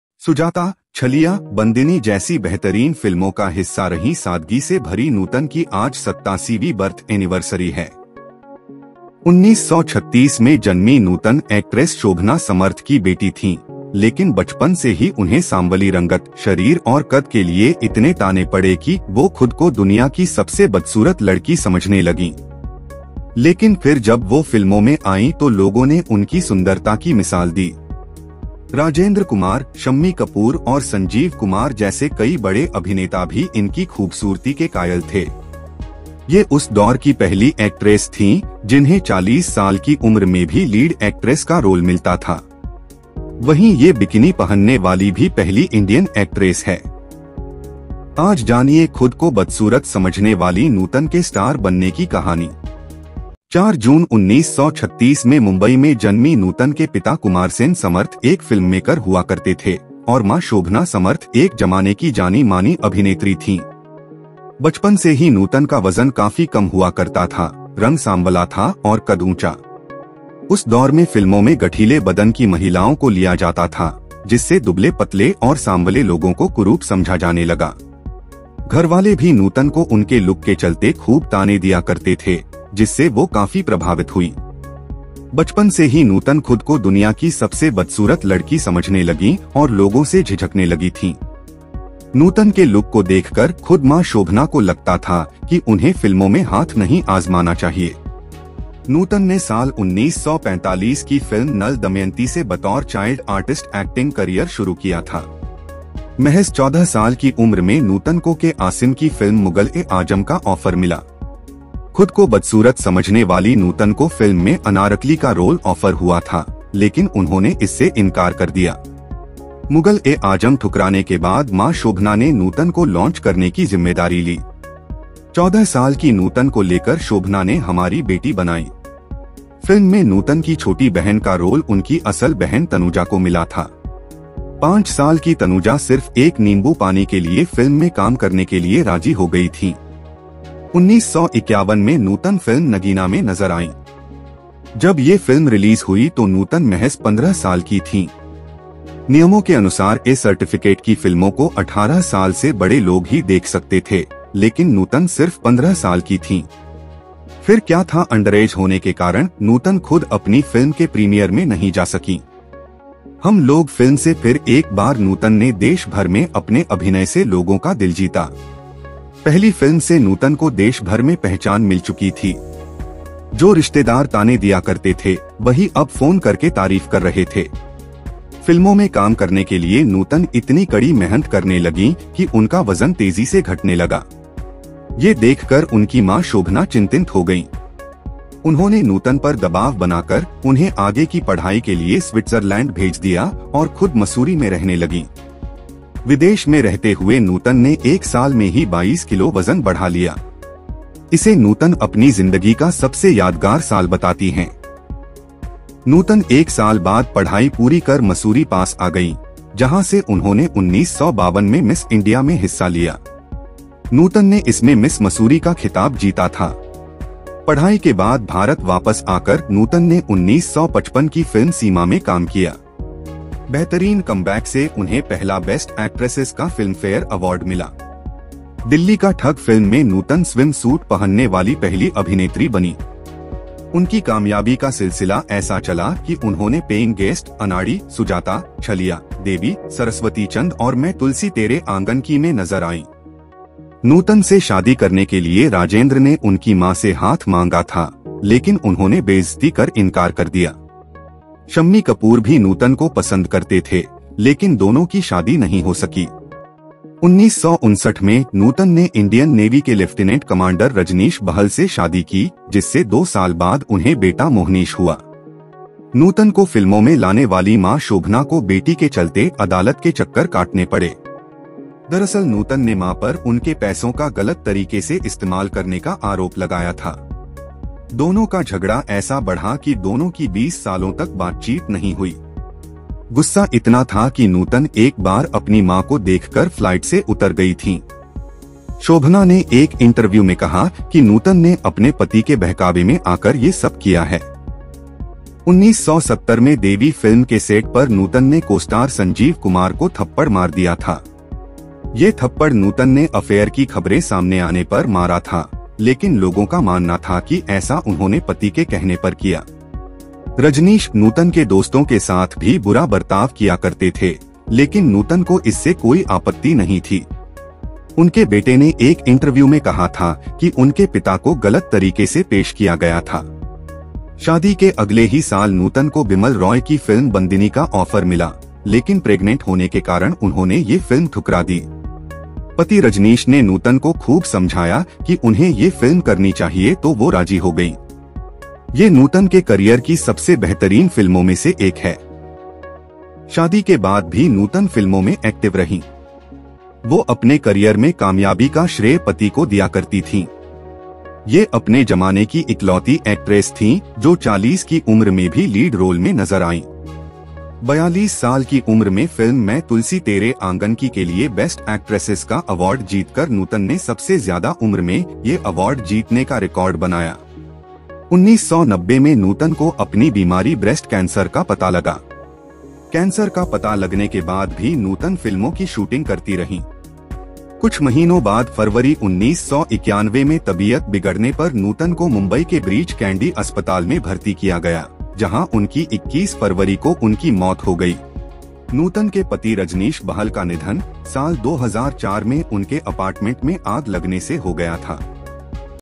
nutan - bigerphy in Hindi . नूतन कि जीवनी | life story | जीवन की कहानी
nutan bigerphy in Hindi ! nutan jivan Parichay.nutan # Bollywood bigerphy

 खुदको सबसे बदसूरत लड़की मानती थीं
नूतन: संजीव 'कुमार को सरेआम मारा था थप्पड़, मां पर केस कर 20 साल नहीं की बात

सुजाता, छलिया, बंदिनी जैसी बेहतरीन फिल्मों का हिस्सा रहीं सादगी से भरी नूतन की आज 87वीं बर्थ एनिवर्सरी है। 1936 में जन्मीं नूतन एक्ट्रेस शोभना समर्थ की बेटी थीं, लेकिन बचपन से ही उन्हें सांवली रंगत, शरीर और कद के लिए इतने ताने पड़े कि वो खुदको दुनिया की सबसे बदसूरत लड़की समझने लगीं। लेकिन फिर जब वो फिल्मों में आईं तो लोगों ने उनकी सुंदरता की मिसाल दी ।

राजेंद्र कुमार, शम्मी कपूर और संजीव कुमार जैसे कई बड़े अभिनेता भी इनकी खूबसूरती के कायल थे। ये उस दौर की पहली एक्ट्रेस थीं जिन्हें 40 साल की उम्र में भी लीड एक्ट्रेस का रोल मिलता था। वहीं ये बिकिनी पहनने वाली भी पहली इंडियन एक्ट्रेस हैं।
आज जानिए खुद को बदसूरत समझने वालीं नूतन के स्टार बनने की कहानी